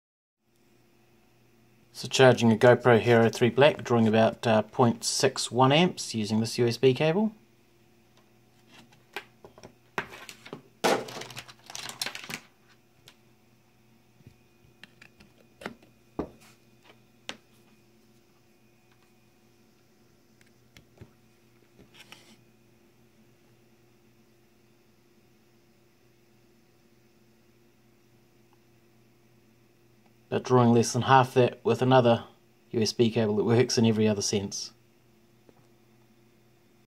Strange USB cable behaviour with Gopro
Confused as to why a battery bank wouldn't charge and power my Gopro, I finally tested the cable (that seemed to work in every way) and one cable only allows half the power of the other(!)